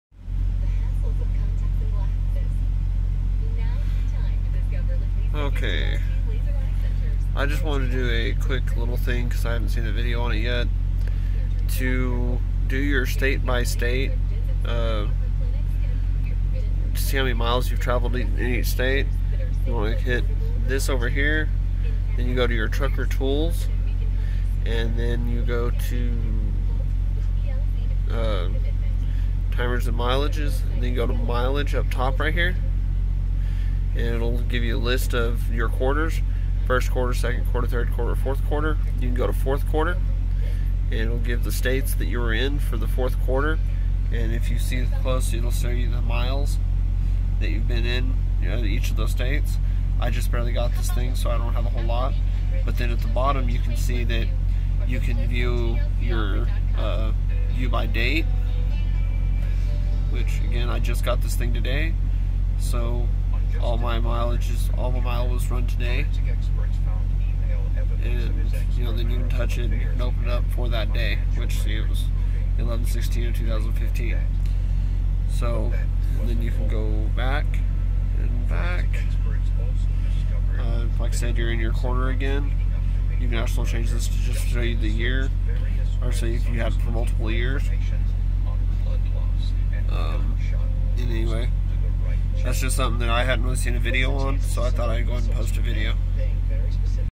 Rand mcnally tnd tablet State by state mileage
A quick tutorial on getting your state mileages.